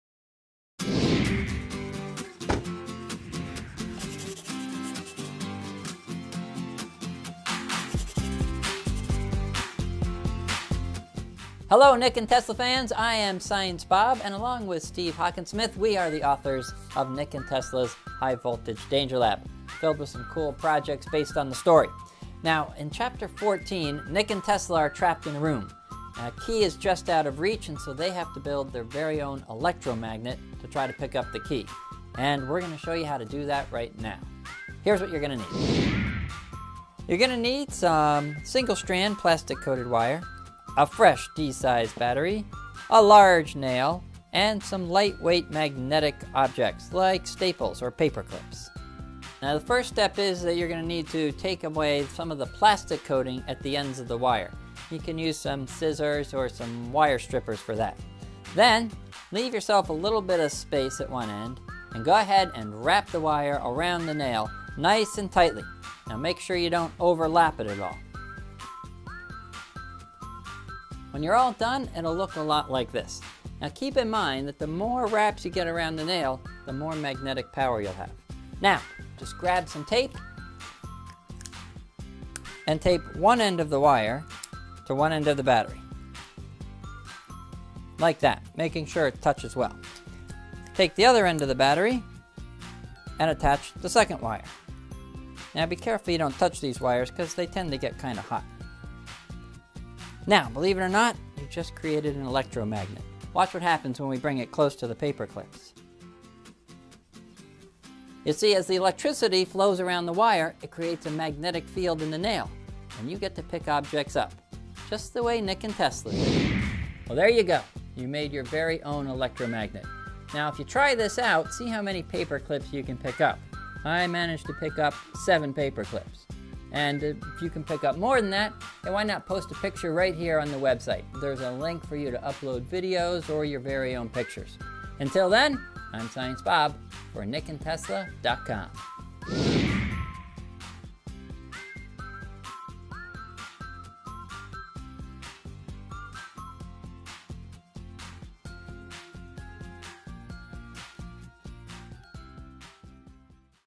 Nick & Tesla: Science Bob's Do-It-Yourself Electromagnet and Picker-Upper
Learn how to make the Do-It-Yourself Electromagnet and Picker-Upper from Nick & Tesla with Science Bob!